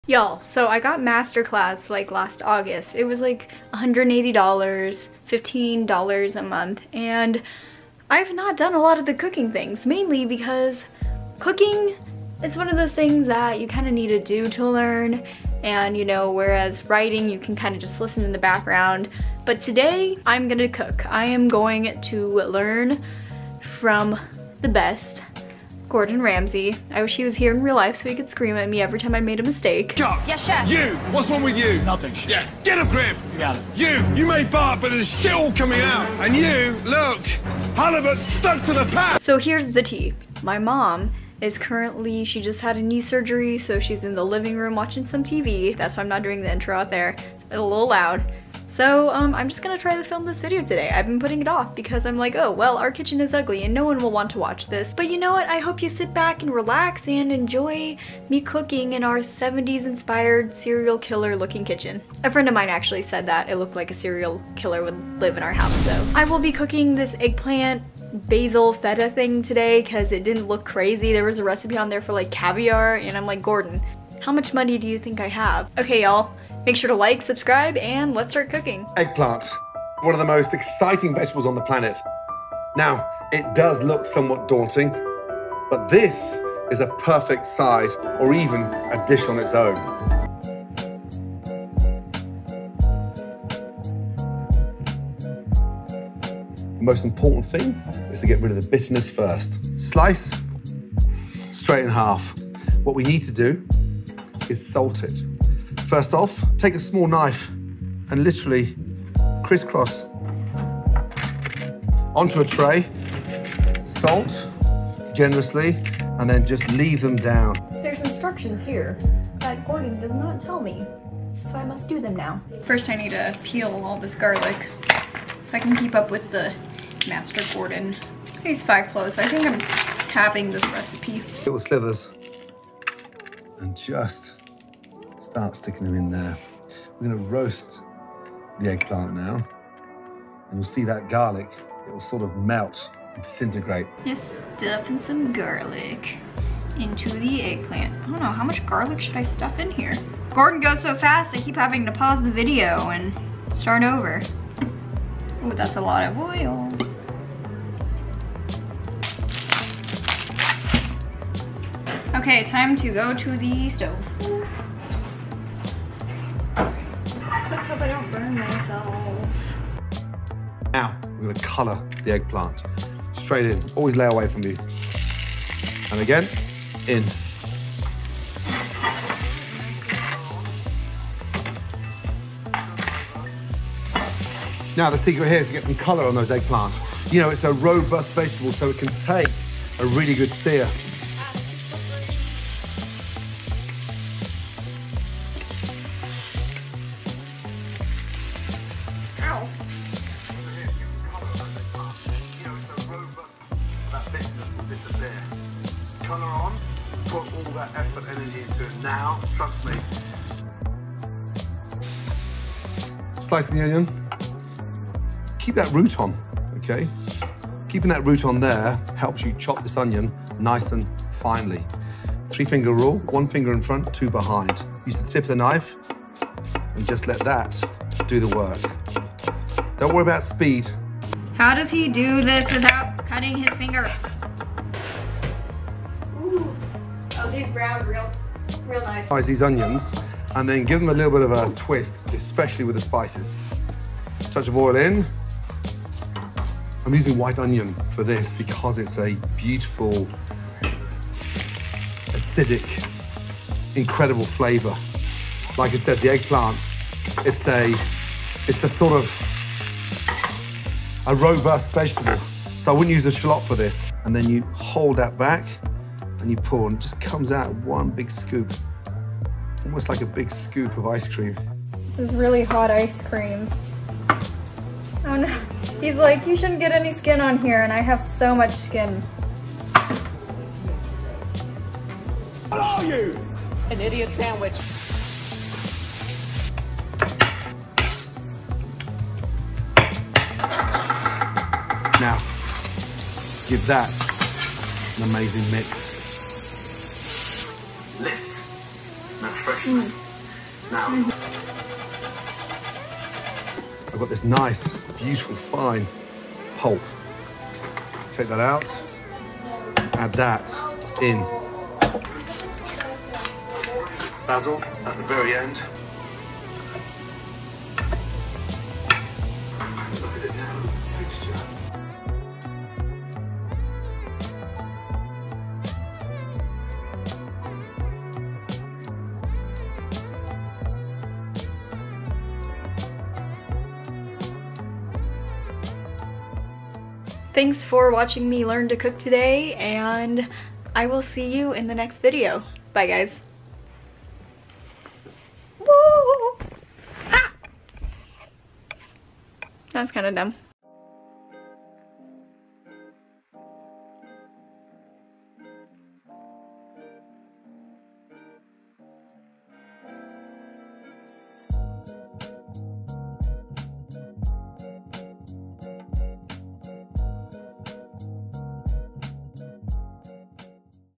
Gordon Ramsay Teaches me How to Cook via Masterclass
I have had Masterclass for a while and never got around to doing much cooking. Well, we are changing that today as I prepare an eggplant dish with my bro Gordon.